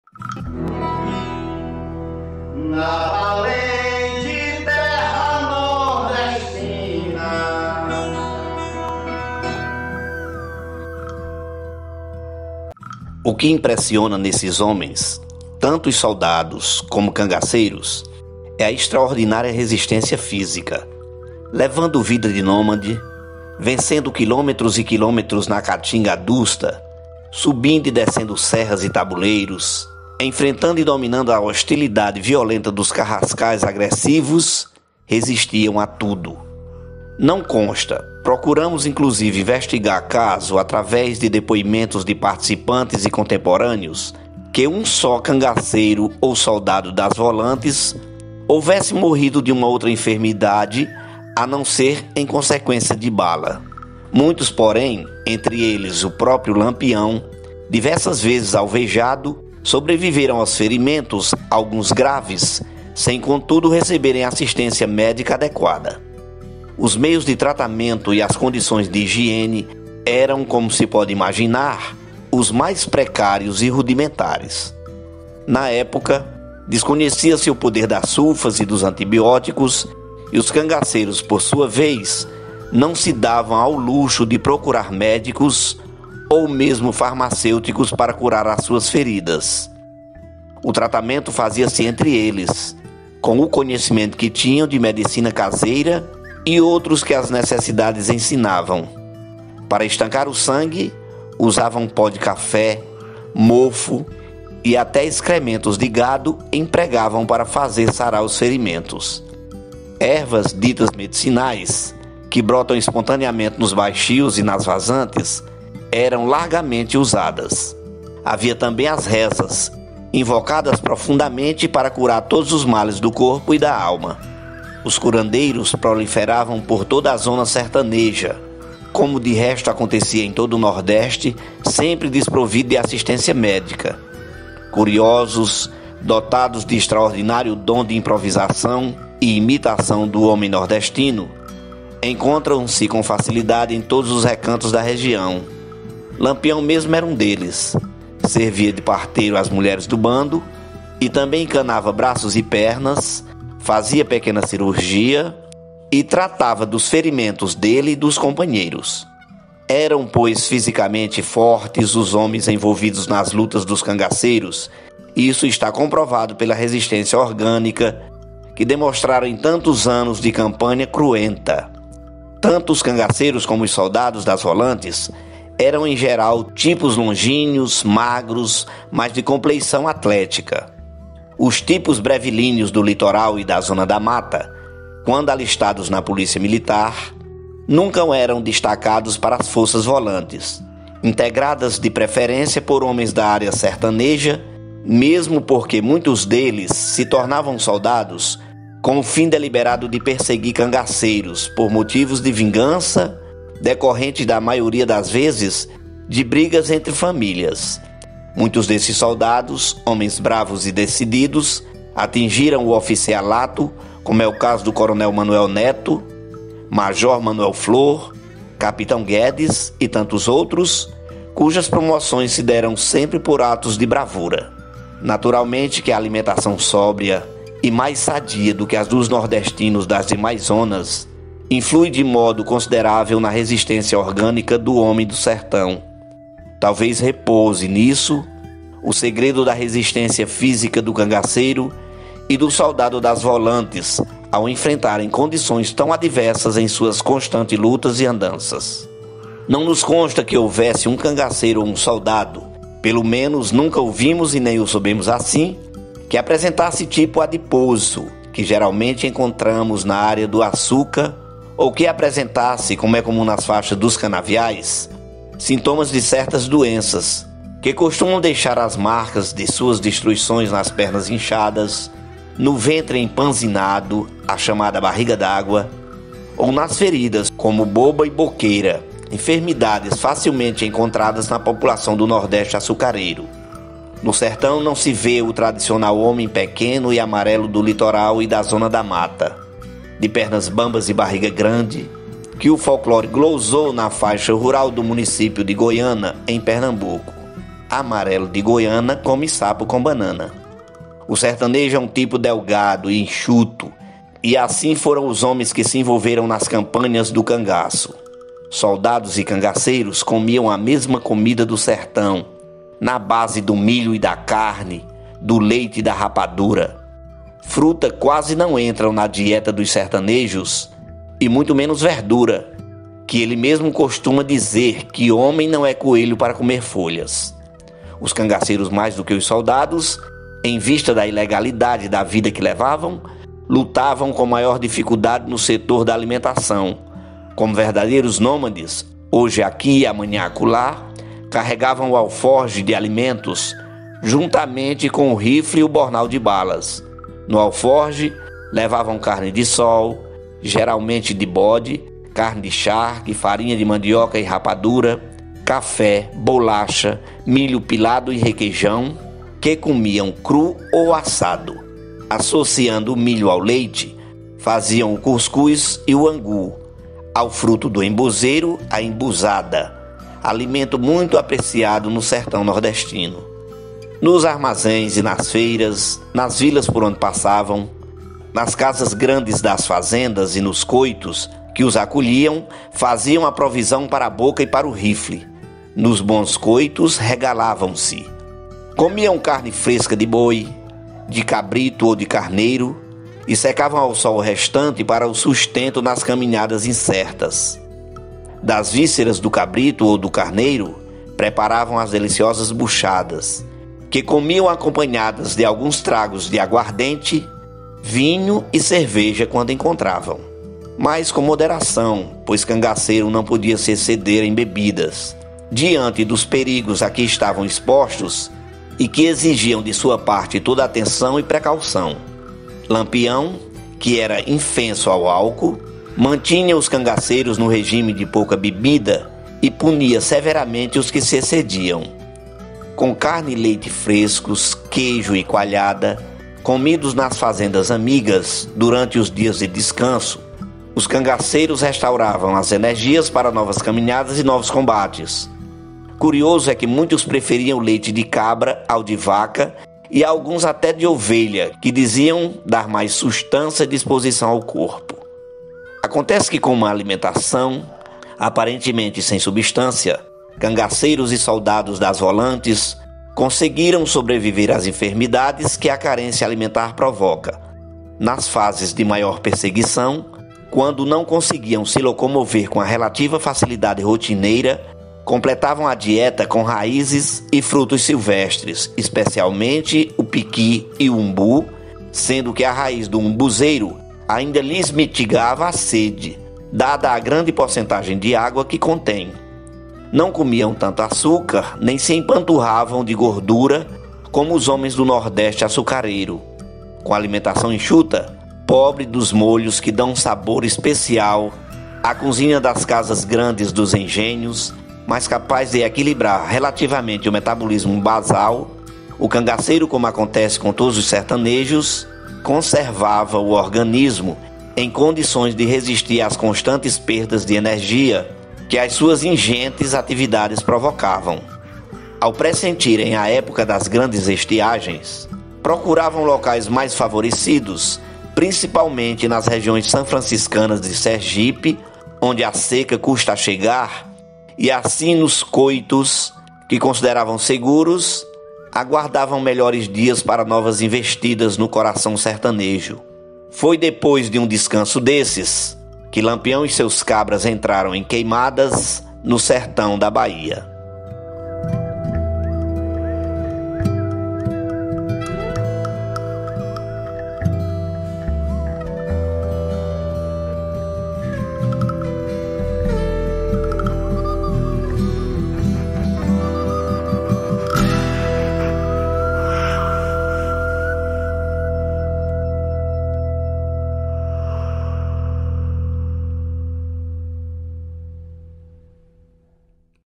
A INCRÍVEL RESISTÊNCIA DE LAMPIÃO, CANGACEIROS E VOLANTES
Eram, pois, fisicamente fortes os homens envolvidos nas lutas do cangaço e isso está comprovado pela resistência orgânica que demonstraram em tantos anos de campanha cruenta. Tanto os cangaceiros como os soldados das “volantes” eram, em geral, tipos longilíneos, magros, mas de compleição atlética. Os tipos brevilíneos do litoral e da zona da mata, quando alistados na Polícia Militar, nunca eram destacados para as forças “volantes”, integradas de preferência por homens da área sertaneja, mesmo porque muitos deles se tornavam soldados com o fim deliberado de perseguir cangaceiros, por motivos de vingança, decorrentes, da maioria das vezes, de brigas entre famílias. Muitos desses soldados, homens bravos e decididos, atingiram o oficialato, como é o caso do coronel Manuel Neto, major Manuel Flor, capitão Guedes e tantos outros, cujas promoções se deram sempre por atos de bravura.

Naturalmente que a alimentação sóbria e mais sadia do que a dos nordestinos das demais zonas, influiu de modo considerável na resistência orgânica do homem do sertão. Talvez repouse nisso o segredo da resistência física do cangaceiro e do soldado das “volantes” ao enfrentarem condições tão adversas em suas constantes lutas e andanças.